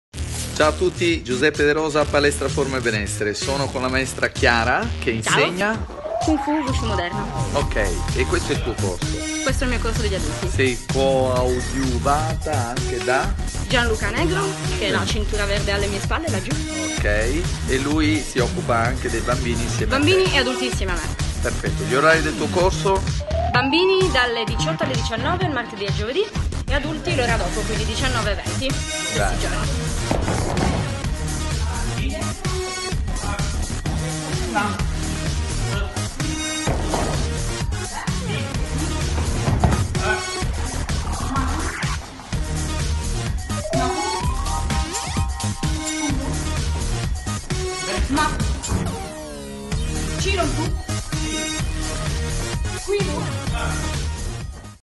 Forma e Benessere Palestra Torino - Kung Fu Eu Shu
In Corso San Maurizio 65 a Torino la Palestra Forma e Benessere propone Corsi di Arti Marziali tutto il giorno! Spot da combattimento, Street Fighting e difesa personale e ovviamente corsi di Kung Fu. 011/836.846